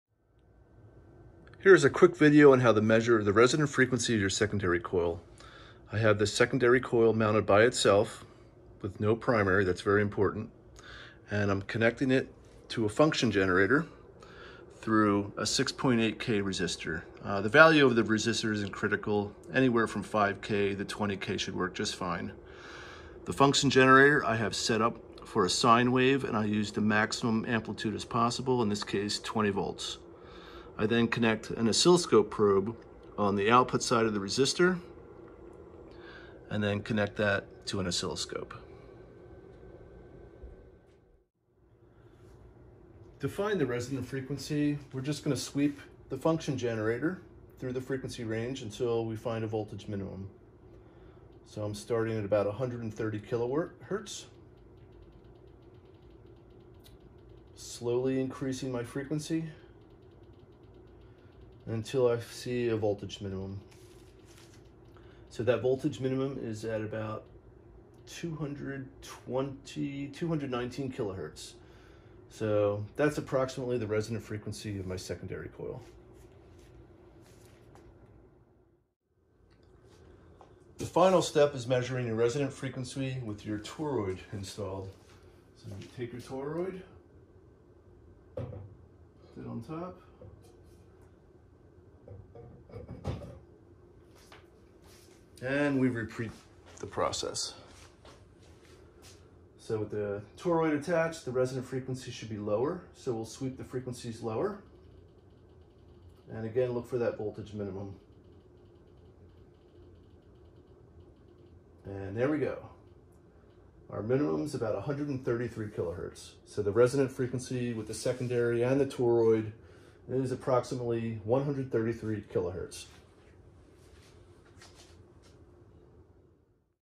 How to Measure the Resonant Frequency of your Secondary Coil
This video shows how to measure the resonant frequency of your Tesla coil's secondary coil using an oscilloscope and function generator.